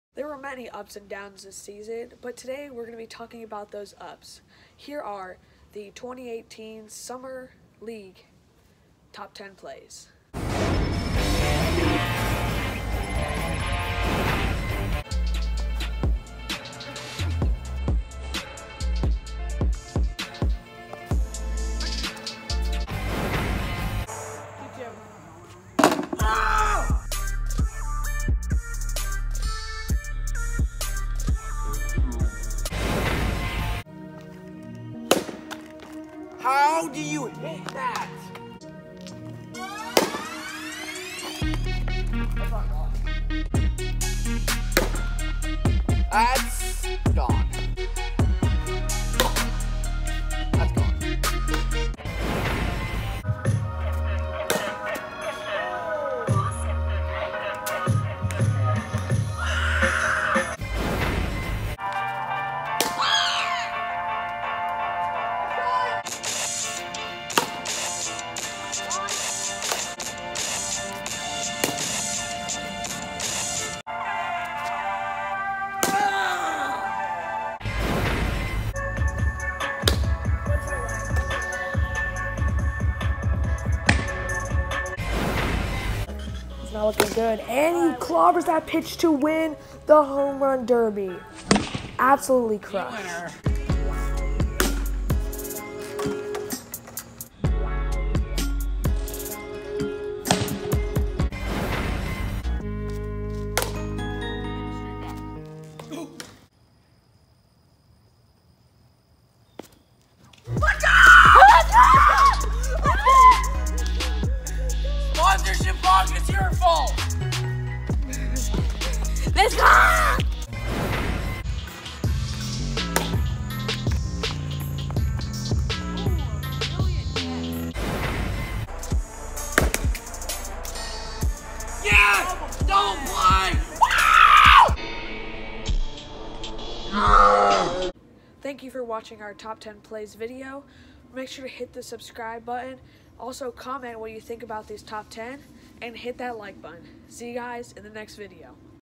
GVBL Summer 2018 Top Ten Plays
Your Summer 2018 Top Ten Plays! These plays were selected by the players, and the plays are absolutely insane! It includes both offensive and defensive plays, but I think that defensive plays are worth more! Congratulations to Josh Y on being our #1 play of the season, and congrats to anyone who made the list! I hope you enjoy this video!
Next video: NOT Top 10 Plays